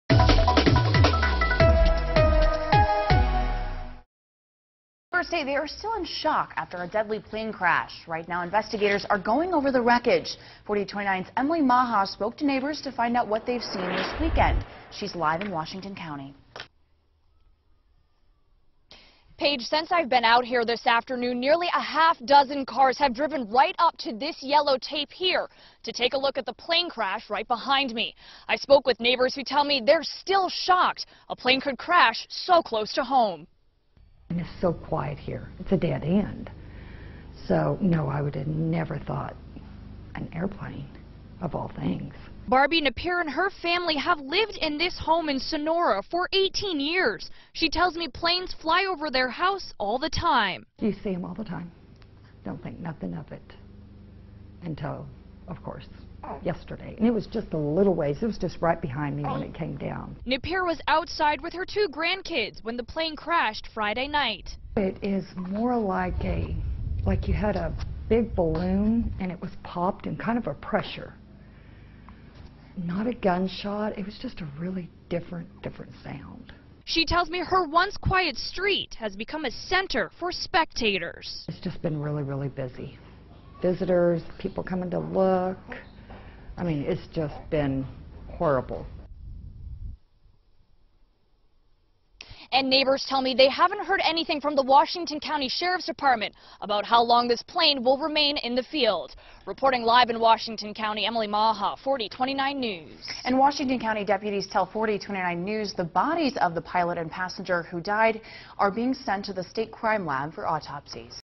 Neighbors say plane crash site drawing in spectators
As investigators look for clues in a deadly plane crash, neighbors can't get the sights and sounds of the crash out of their minds.